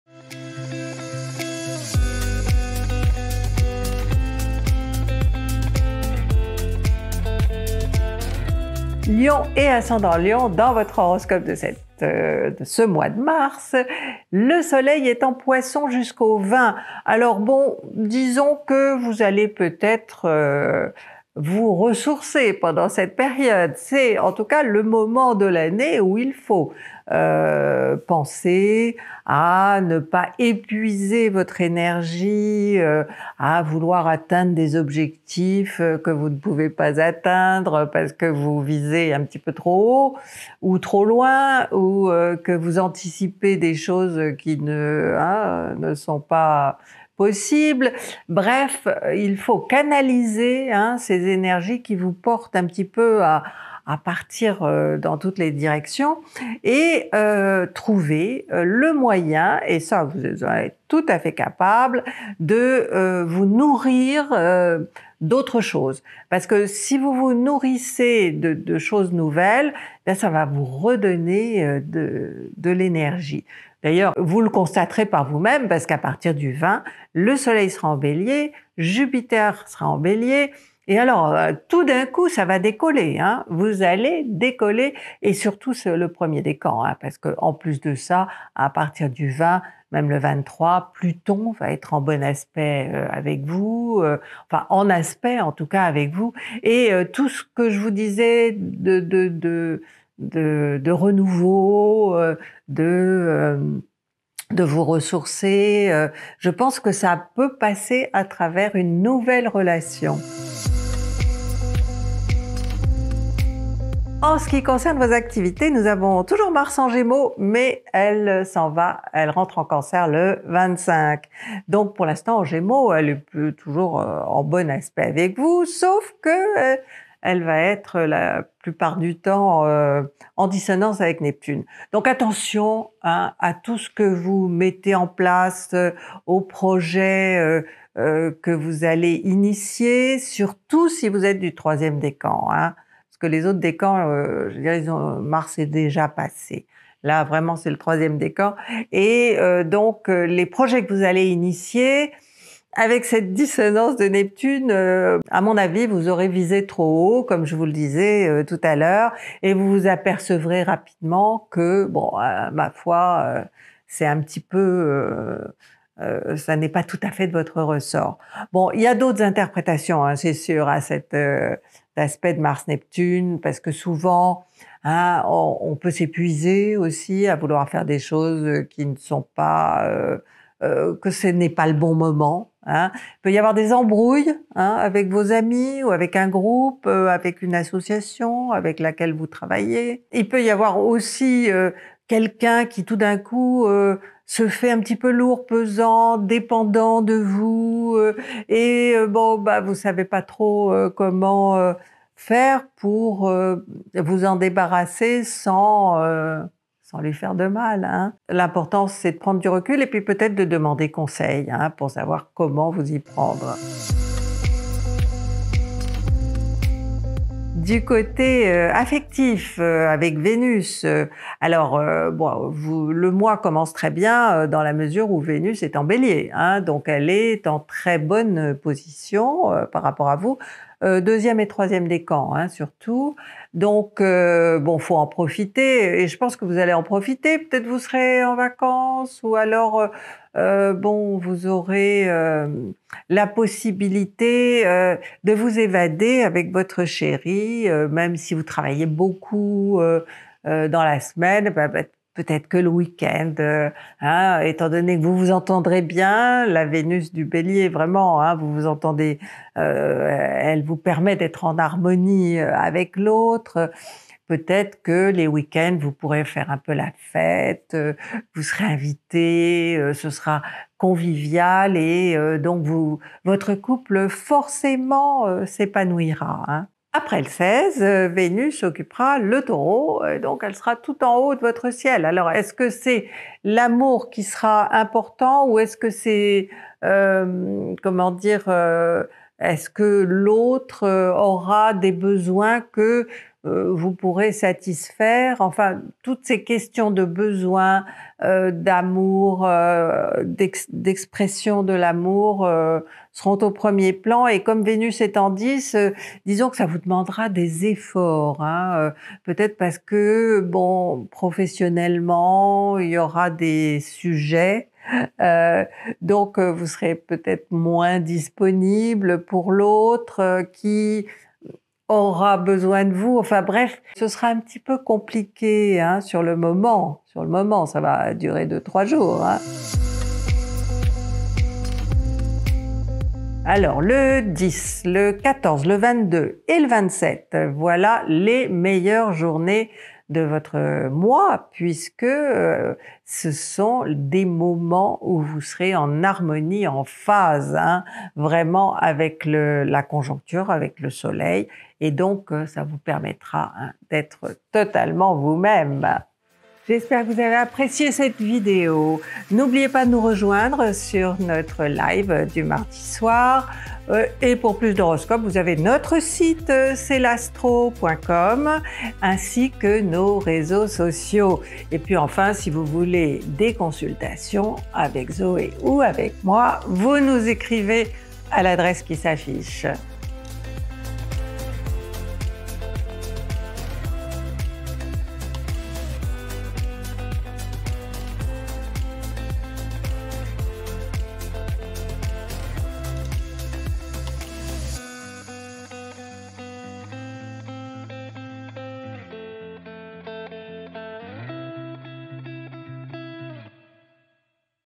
Horoscope Lion ♌️ Mars 2023 par Christine Haas
🌞 horoscope gratuit du signe du lion pour le mois de mars #2023 avec des conseils en astrologie et en psychologie.

⭐️ Retrouvez votre signe:
0:00 Introduction
0:08 Ambiance
1:50 Activités
3:33 Vie affective
5:29 Vos jours top
5:56 Bye bye